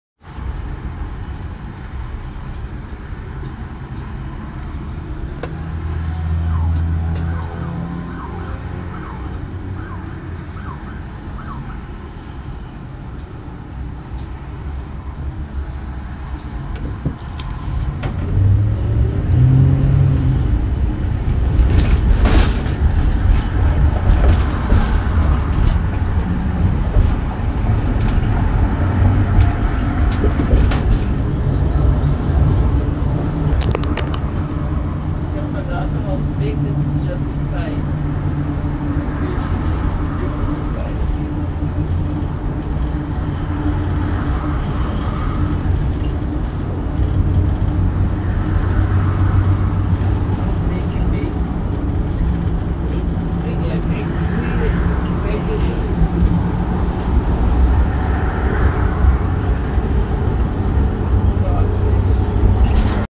Cleanup of accident on the Belt Parkway East this morning
Cleanup of accident on the Belt Parkway East